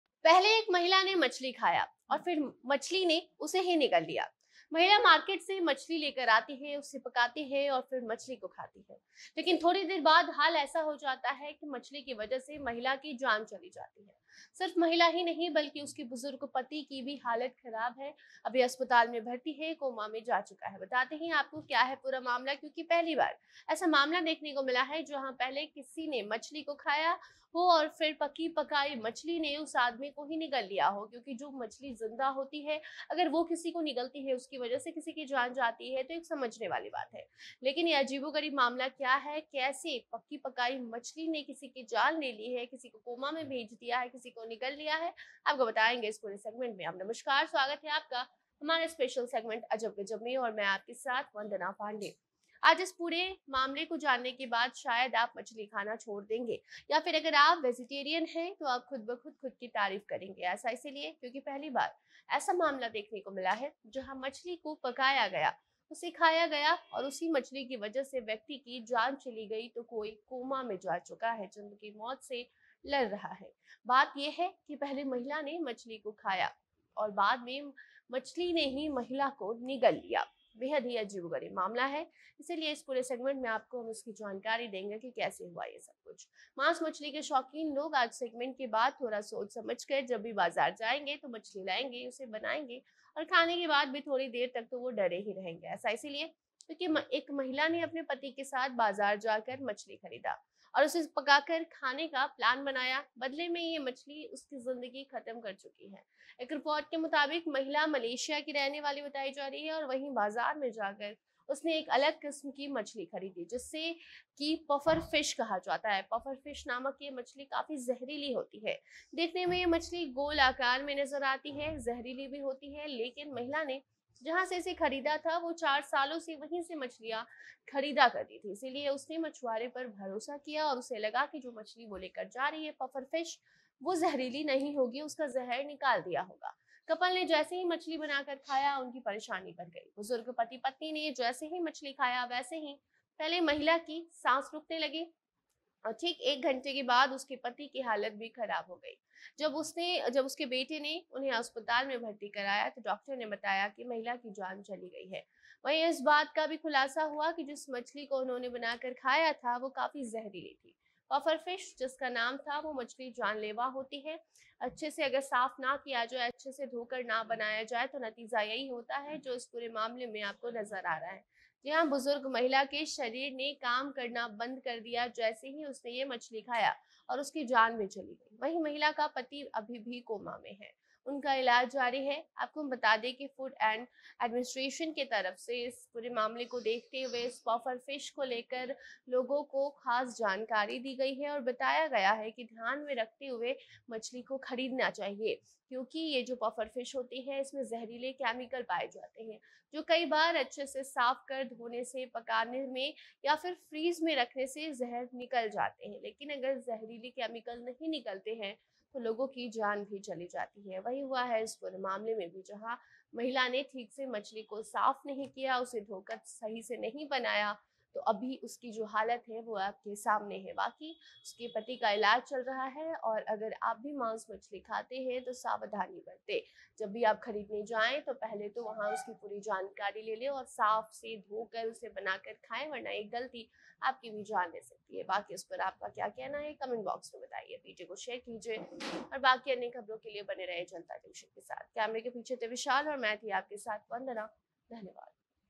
अजब गजब: मछली ने बुजुर्ग को किया परेशान,पहले महिला ने मछली खाया फिर मछली ने उसे निगल लिया!
अजब गजब: बुजुर्ग महिला ने जैसे ही मछली पकाकर खाया हालत हुई खराब,मछली को निगलना भारी पड़ गया,पति-पत्नी की हालत हुई खराब!